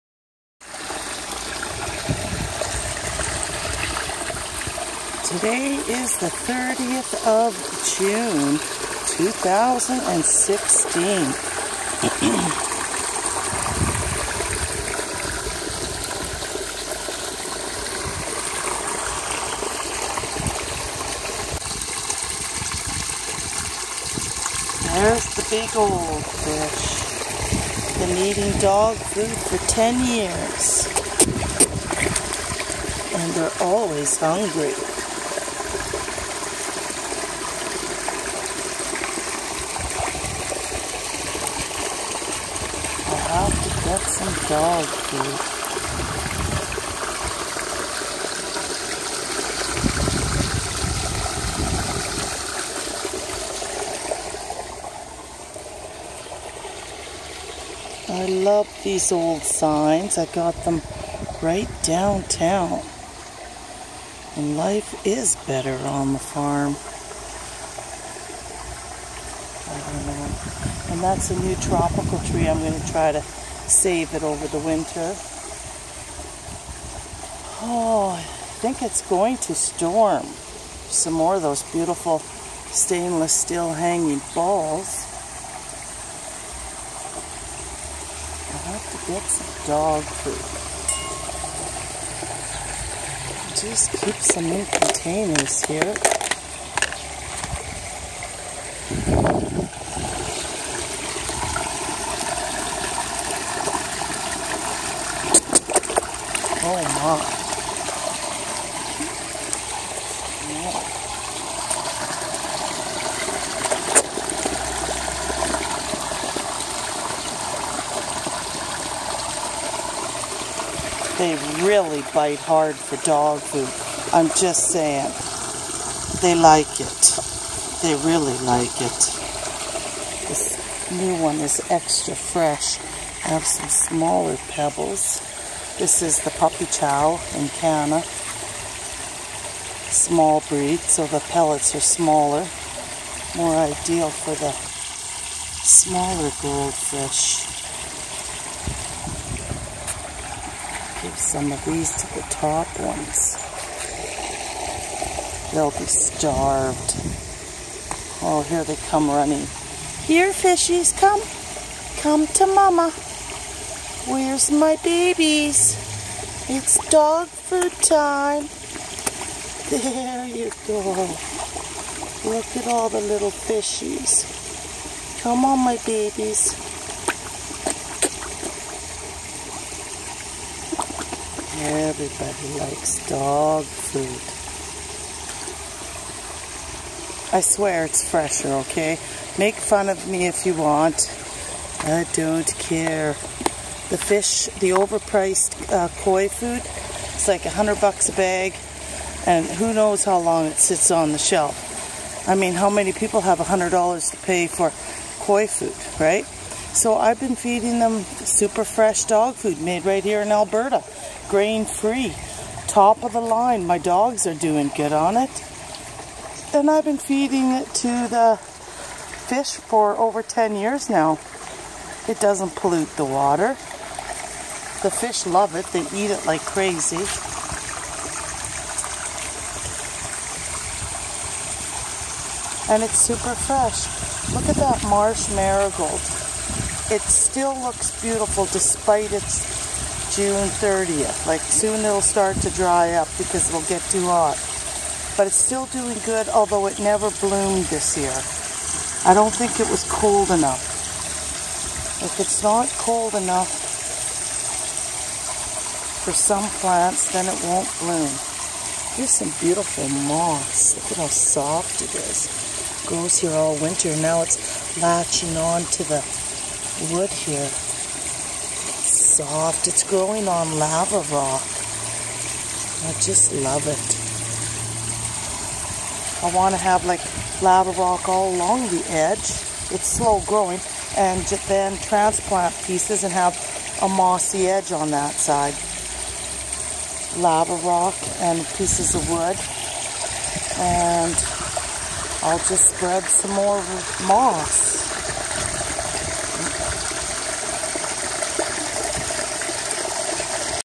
Hungry and they bite very hard for DOG FOOD in Canada!
Two big 25 year old Koi are biting very hard for fresh "grain free" locally made (Alberta Canada) high quality dog food! They have been eating it for at least 10 years now. Ponds are just beautiful today.